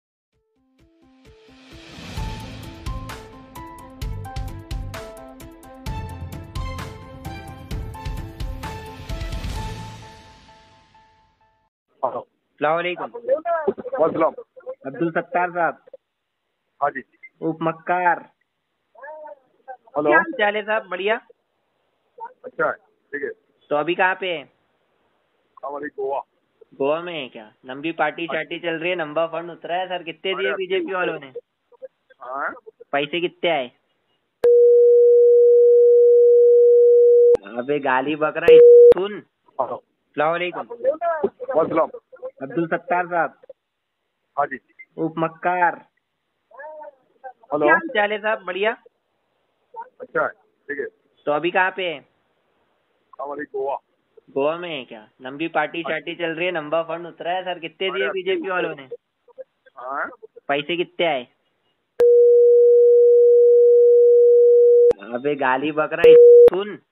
पैसे किती मिळाले विचारताच शिवीगाळ; शिंदे गटातील आमदारांची काॅल रेकॉर्डिंग व्हायरल
VIDEO | पैसे किती मिळाले विचारताच शिवीगाळ; अब्दुल सत्तारांची कॉल रेडॉर्डिंग व्हायरल